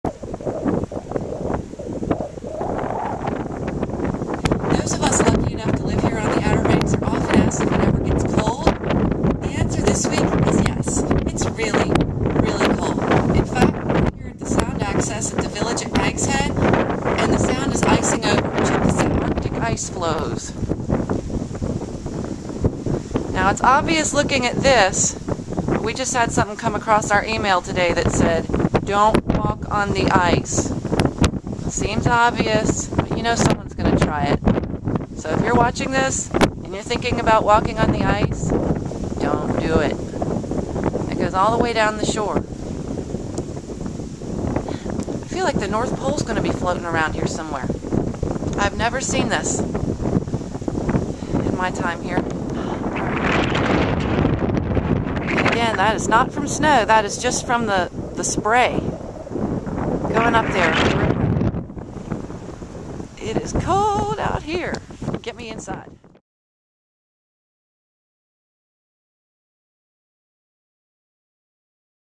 Baby It's Cold in Nags Head!
January 5 actually feels like January this year! The Sound ices up in the Village at Nags Head.